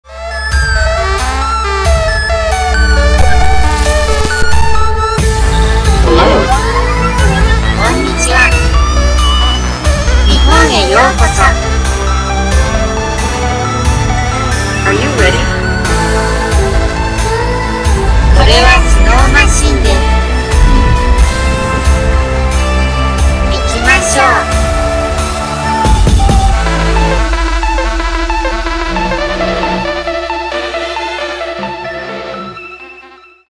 Snow Machine Japan | Day 1 Highlights 2020
Welcome to Snow Machine! A four-day alpine music festival taking place in Japan's largest ski destination of Hakuba.